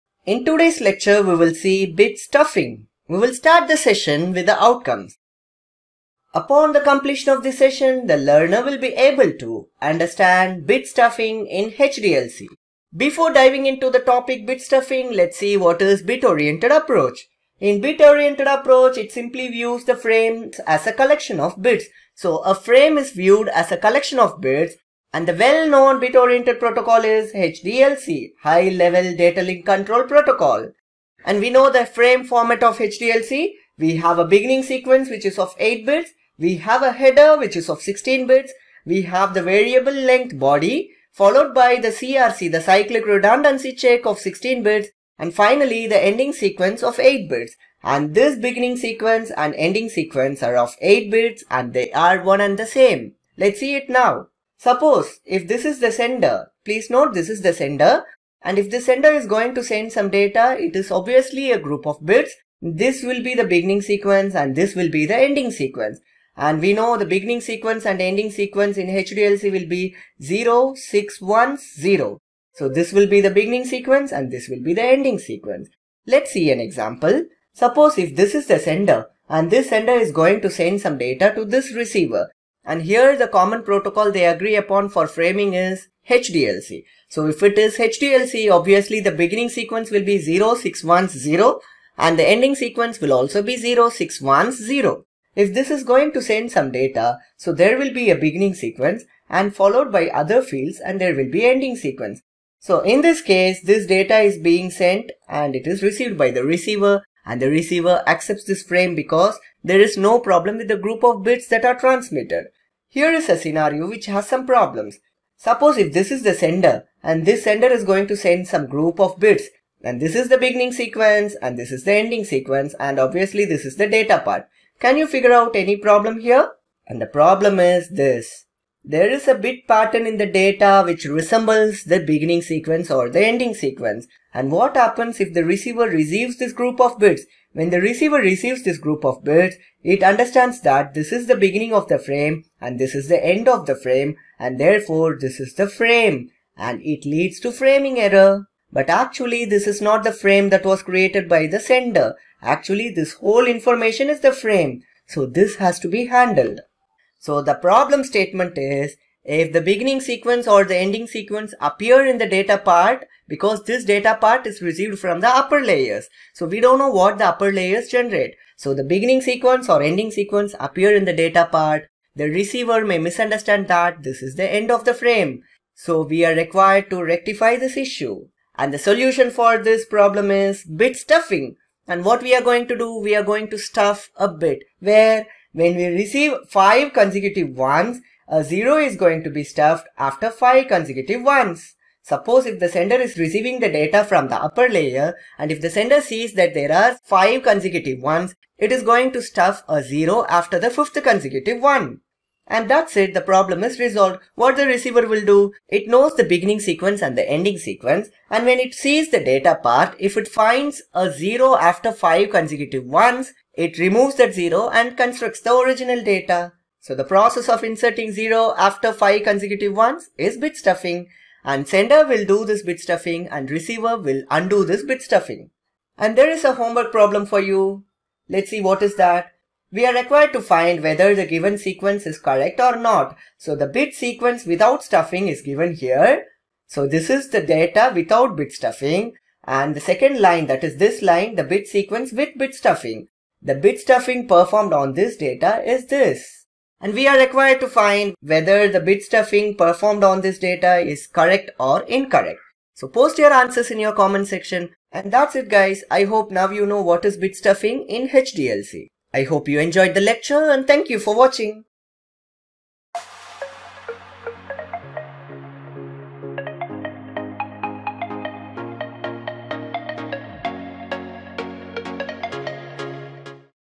Bit Stuffing
Computer Networks: Bit Stuffing in Computer Networks
Topics Discussed:
1) Frame format in HDLC.
2) Framing error.
3) Bit stuffing in HDLC.